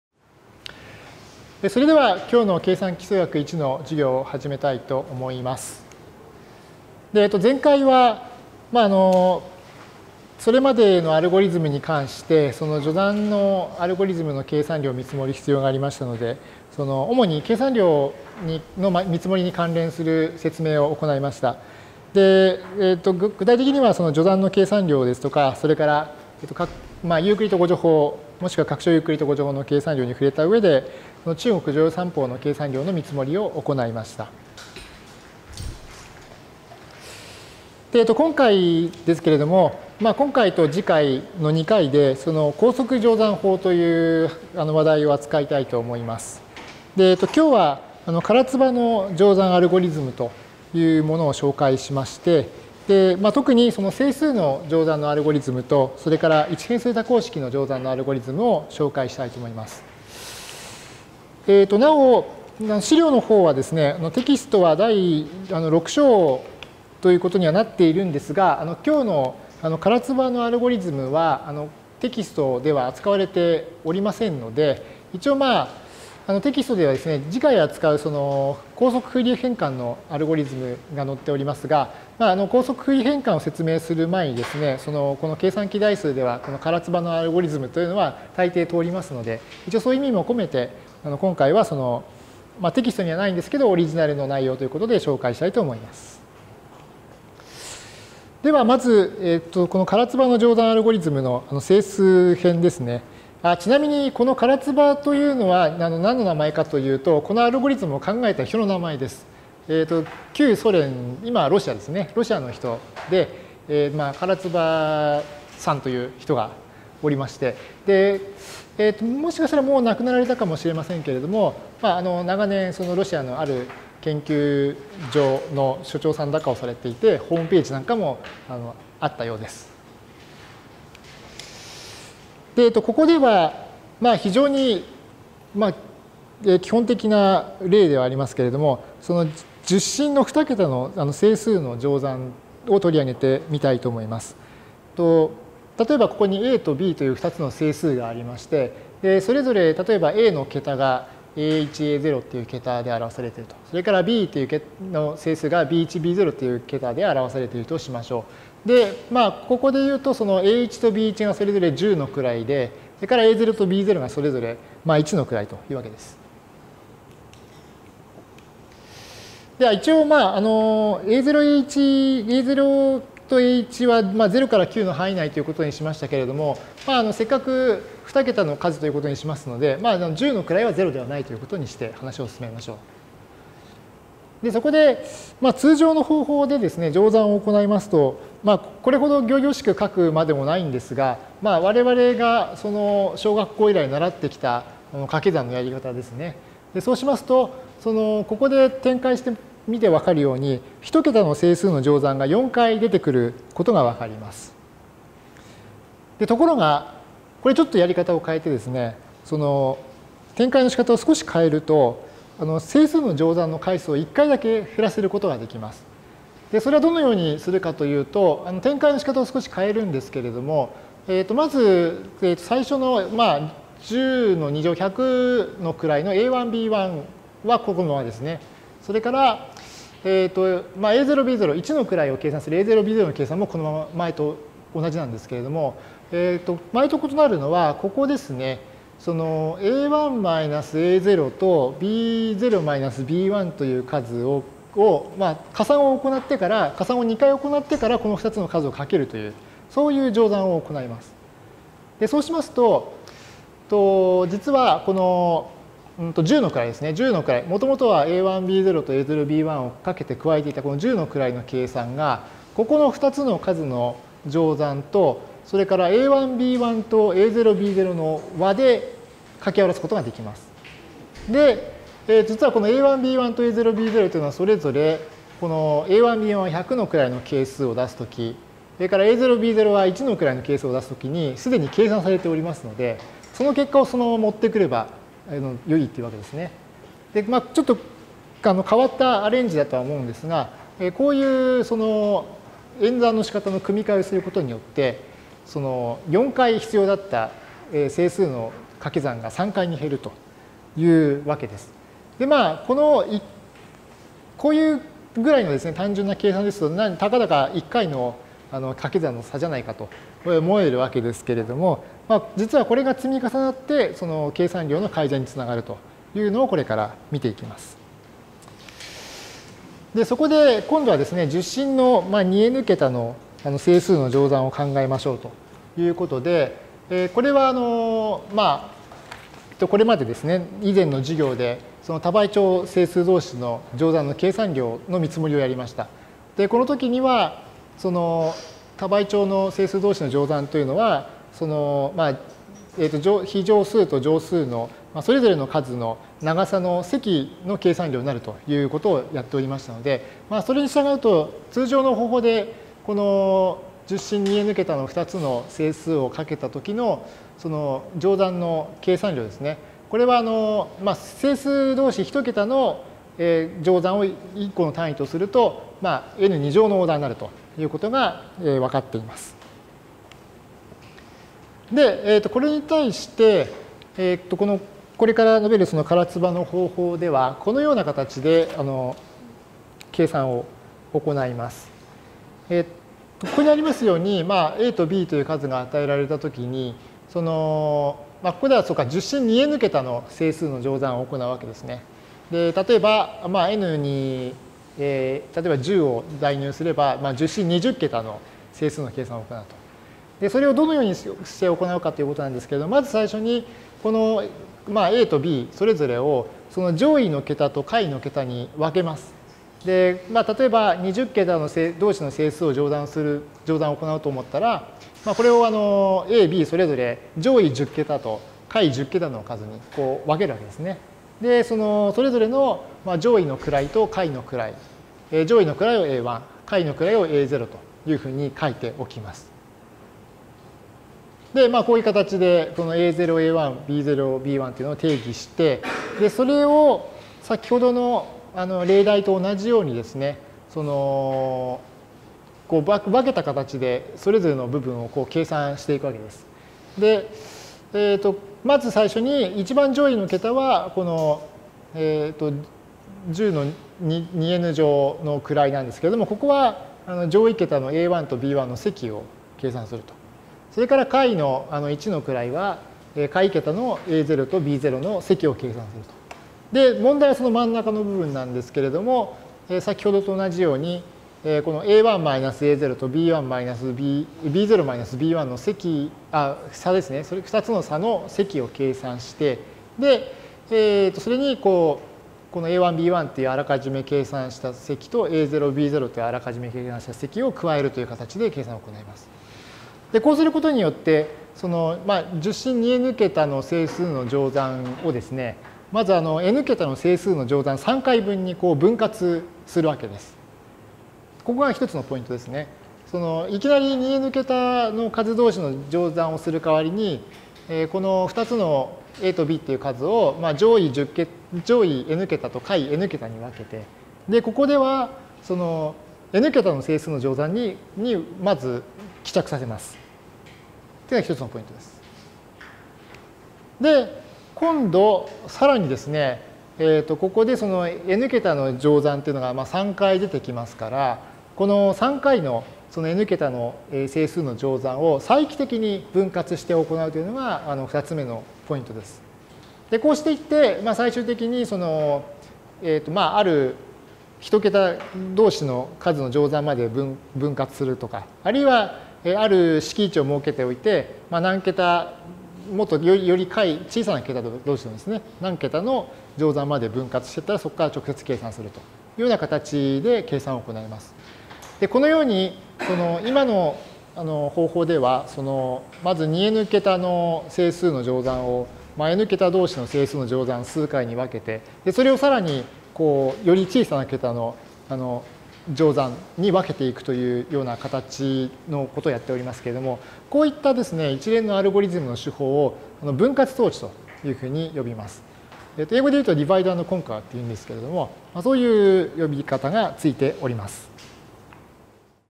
計算機数学 I 2019 (14-1) 整数に対するKaratsubaの乗算アルゴリズム
整数に対するKaratsubaの乗算アルゴリズム．

照井 章. 筑波大学 理工学群 数学類. 計算機数学I. 筑波大学 1E401.